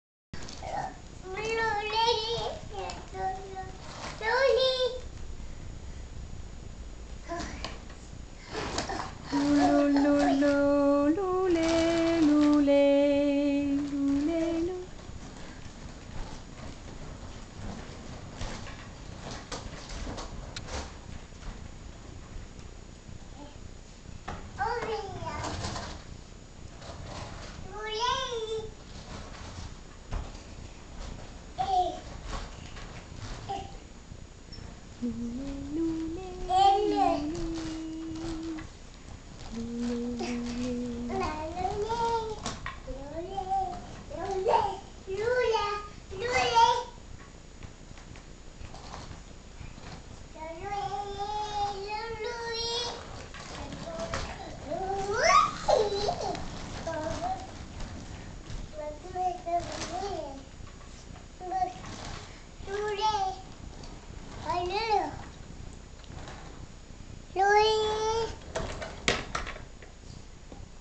Midas is singing
Midas sings while walking around.